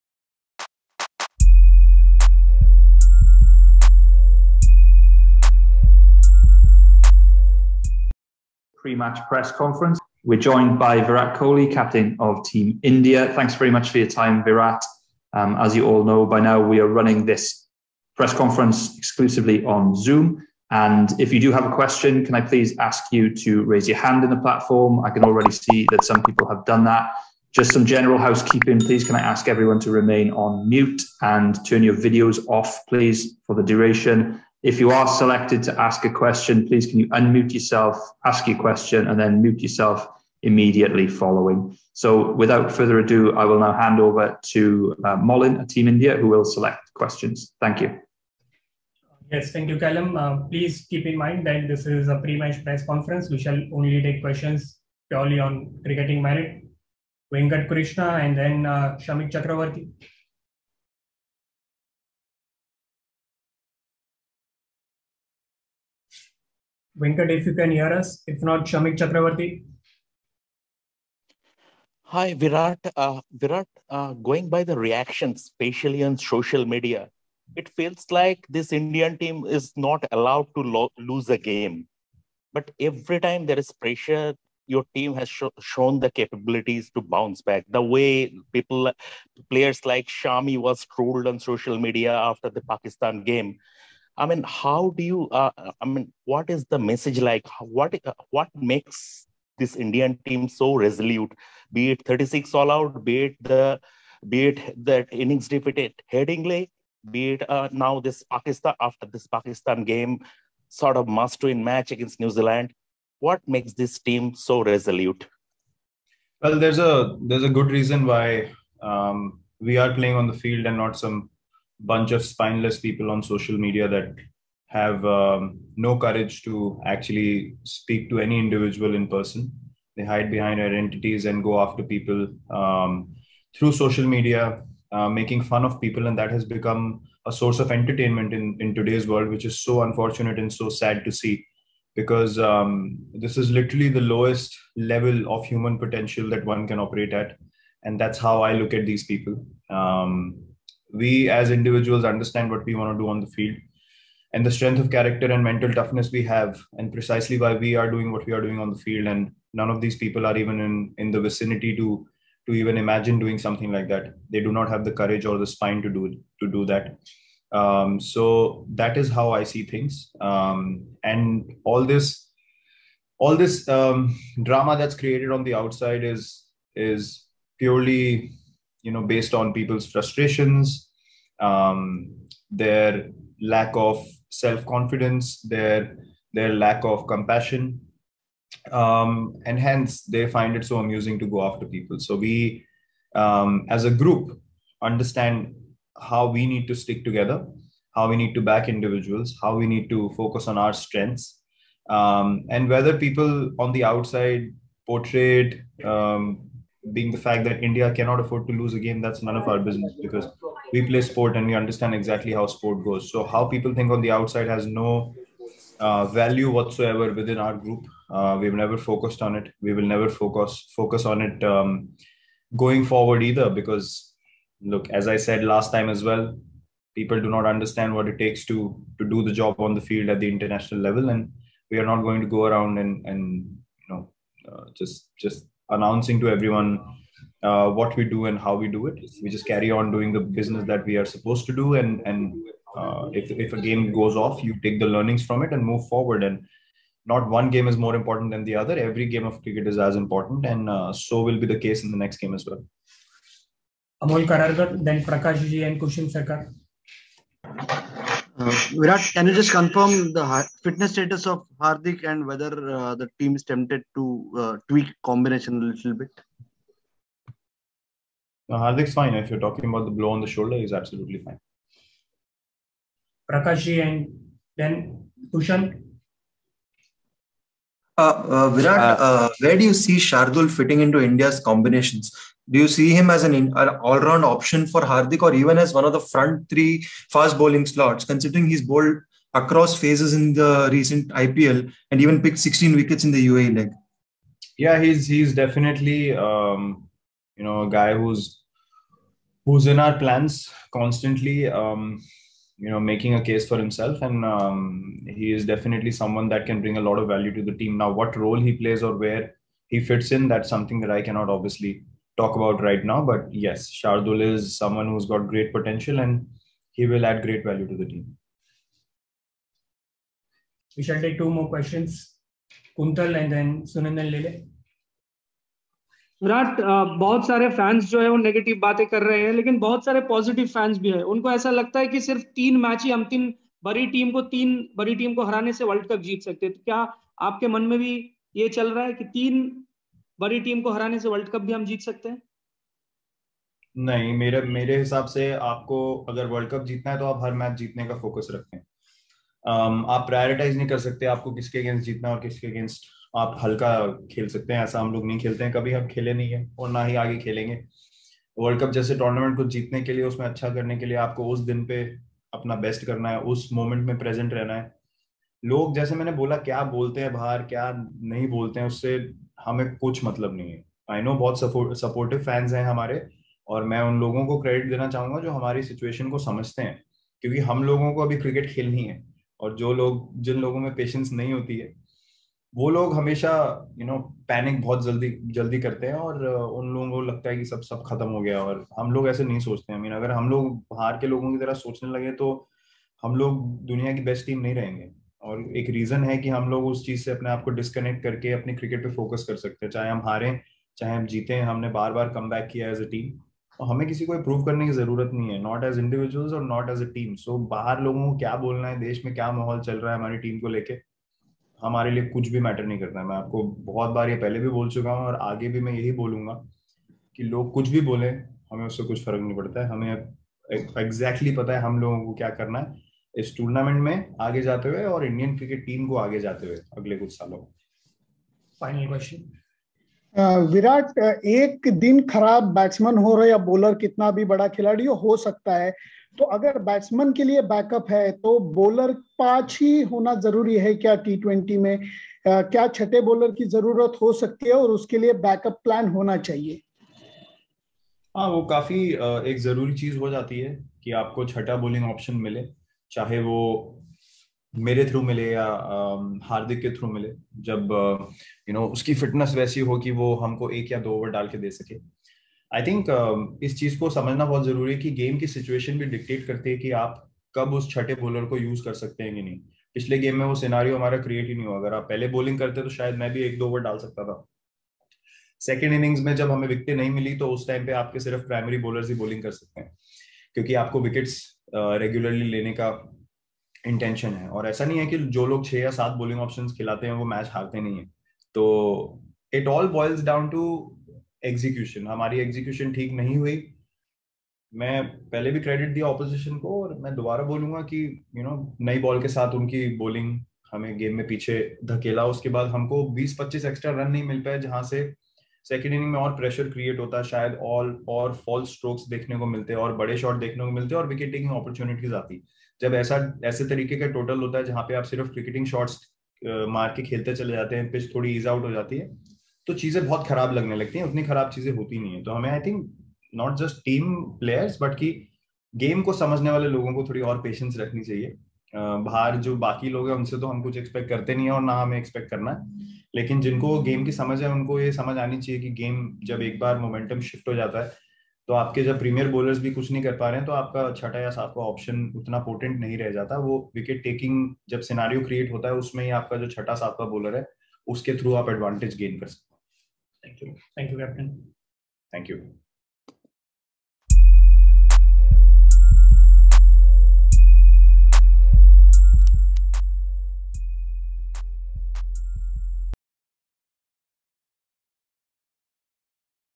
Before Next Game | Pre-Match zoom press conference By Virat kohli
Credit - ICC
Video chording - MK Marsh (GoproSport Team)